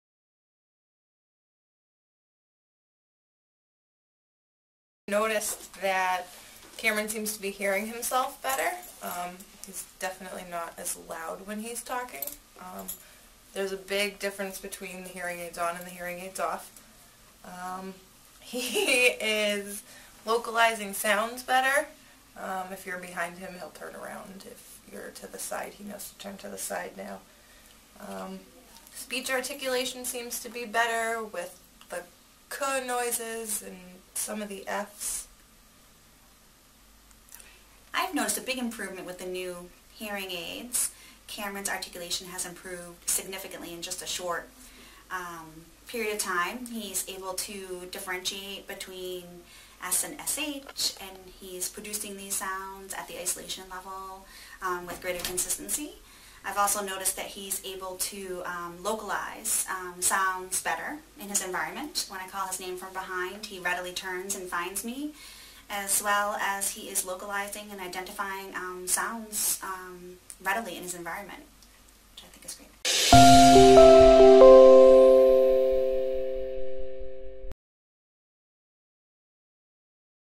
Cameron's story video - Sensei SP
After being fitted with Oticon Sensei Super Power hearing aids, Cameron can now hear and differentiate between ‘K’, ‘F’, ‘S’ and ‘SH’ sounds. Hear his mother and his speech therapist explain the improvement in speech articulation.